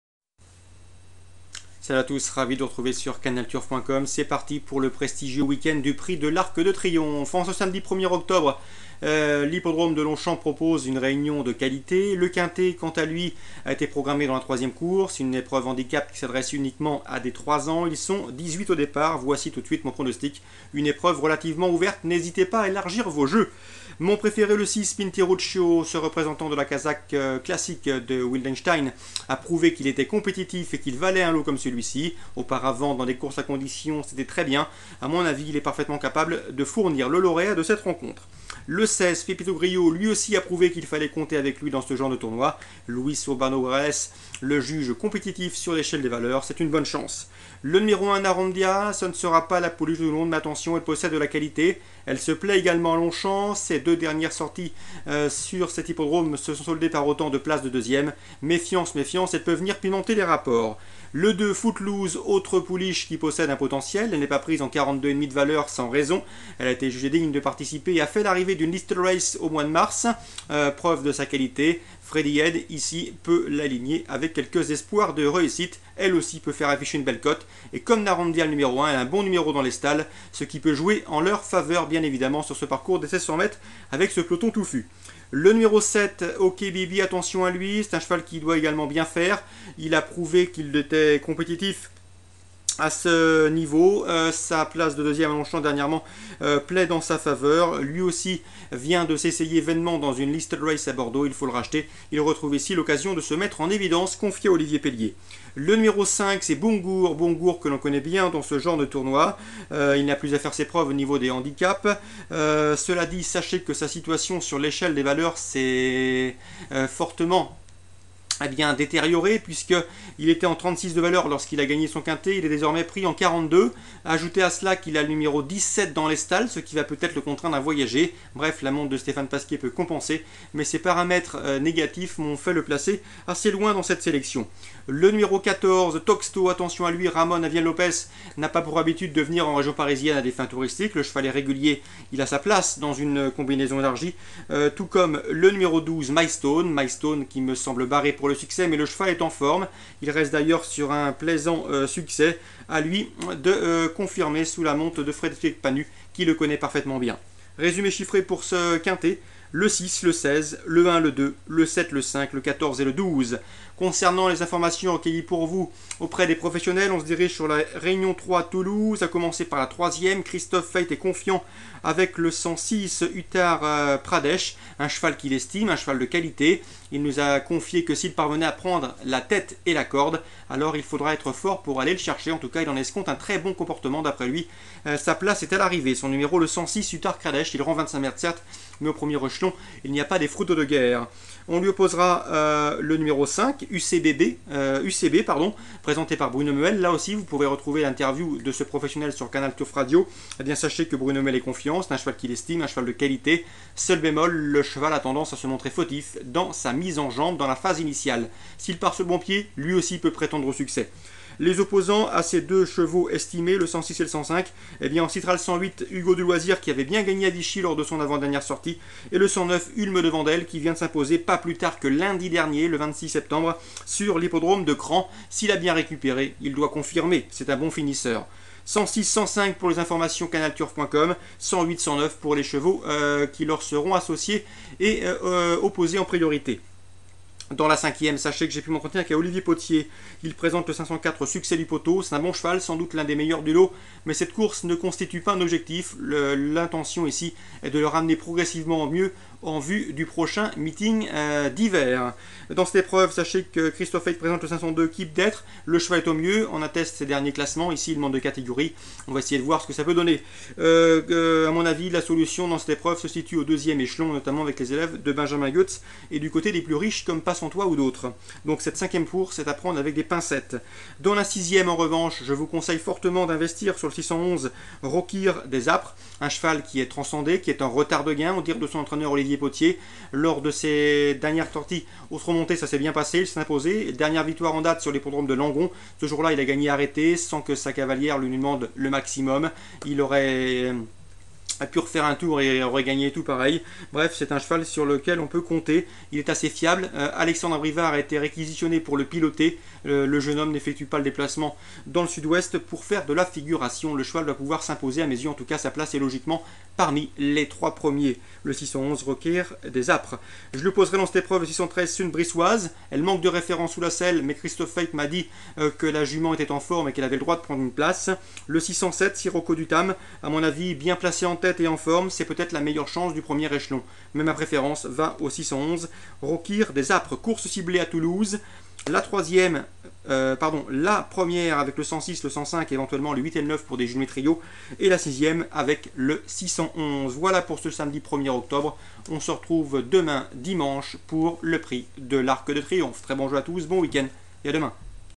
Canalturf TV - Samedi 1 Octobre 2011 - PRIX QATAR RACING & EQUESTRIAN CLUB - LONGCHAMP
Réunion 1 Course 3 - PLAT - 1600m - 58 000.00€